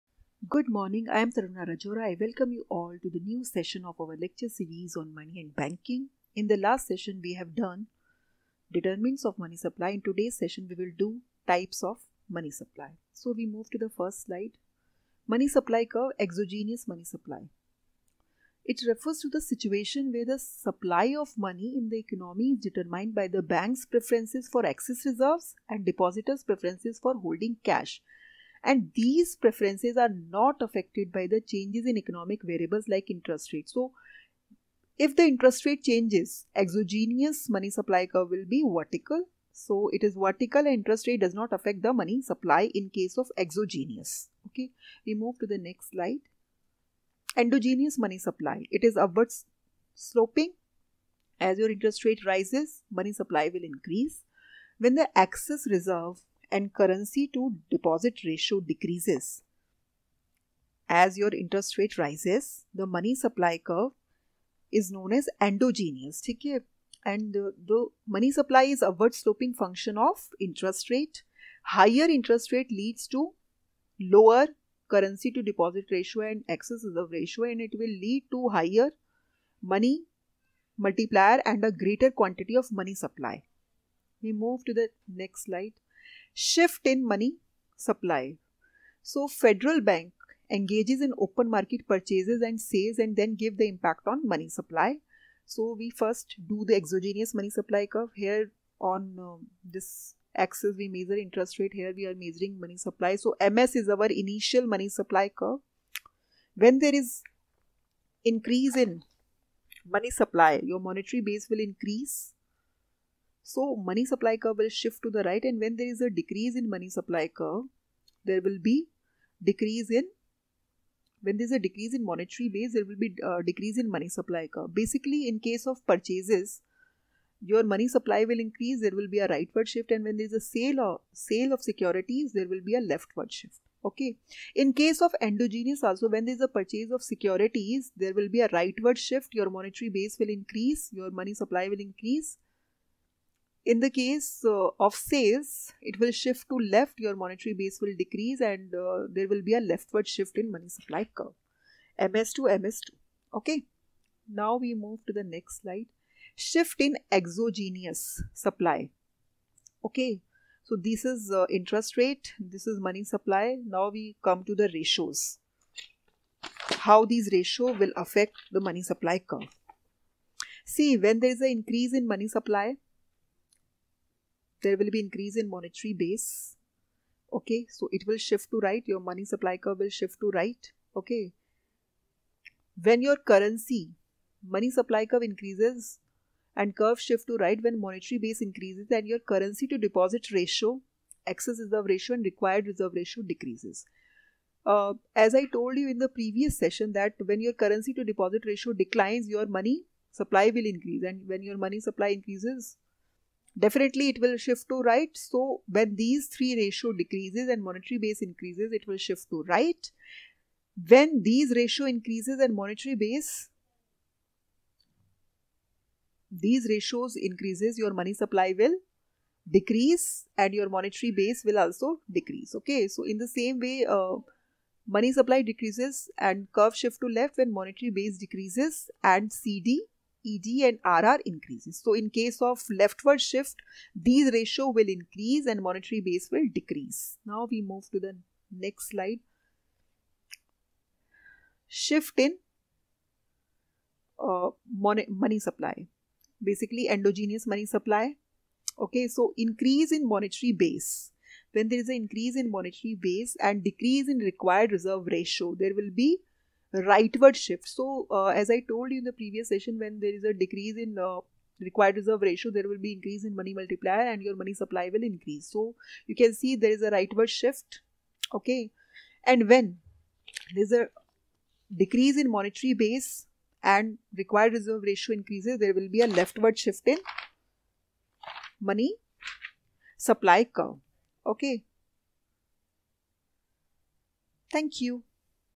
Exogenous & Endogenous Money Supply | GE | Economics
In this video we will talk about the exogenous and endogenous money supply. It also explains the shift in the curves due to monetary base, currency deposit ratio, excess reverse ratio, and required reserve ratio.